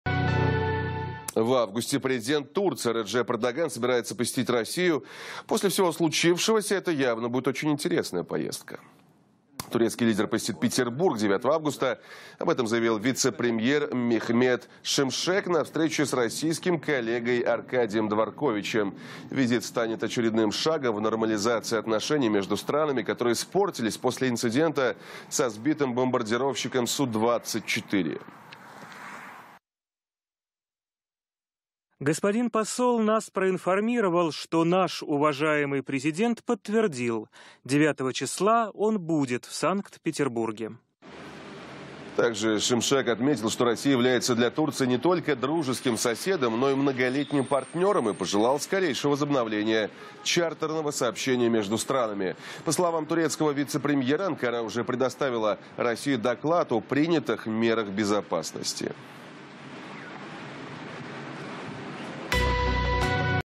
В августе президент Турции посетит Россию
9 августа Реджеп Эрдоган приедет в Санкт-Петербург